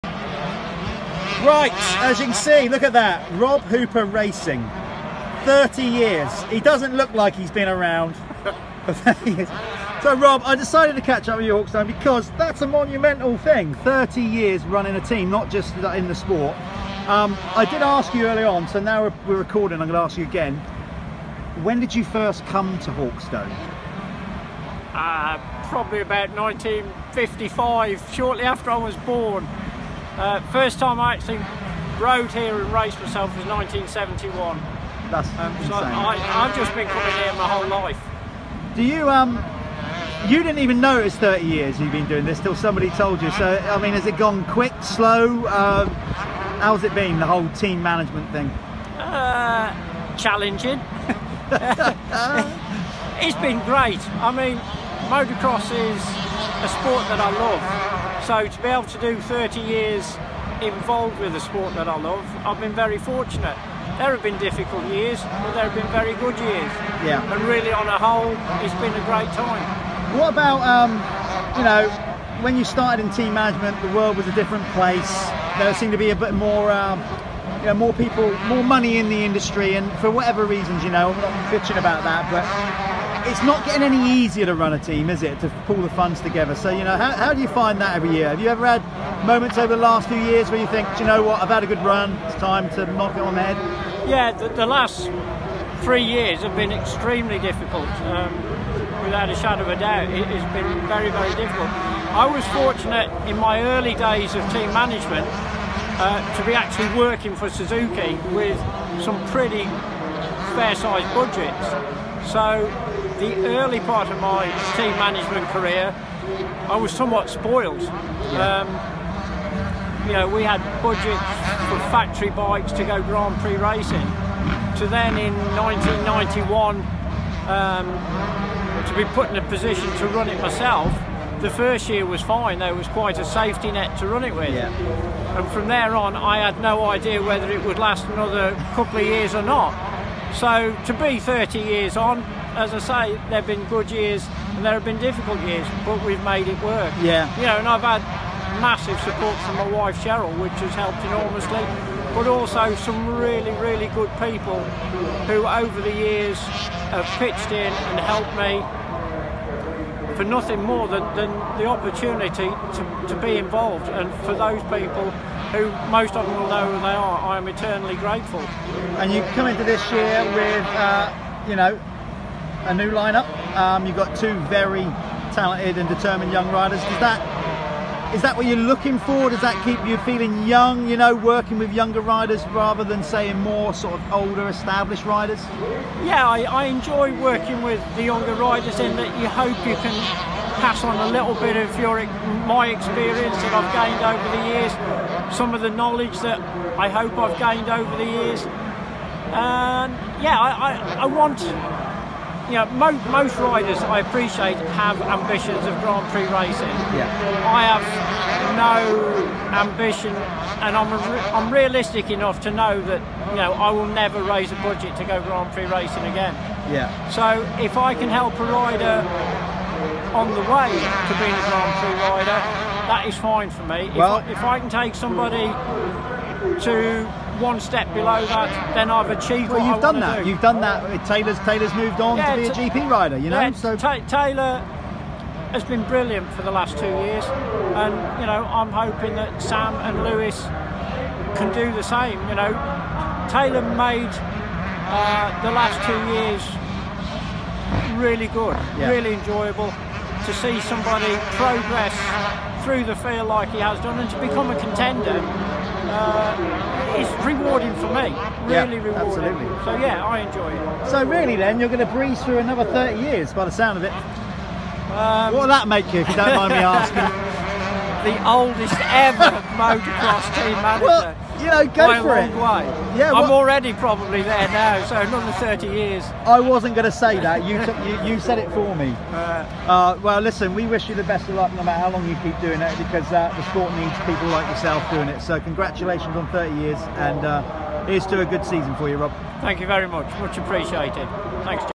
Rob Hooper - 30 years running a Motocross team
We catch up with Rob Hooper who has been running a British Motocross team for 30 years and continues to develop young talent through his Rob Hooper Racing Yamaha team in the UK.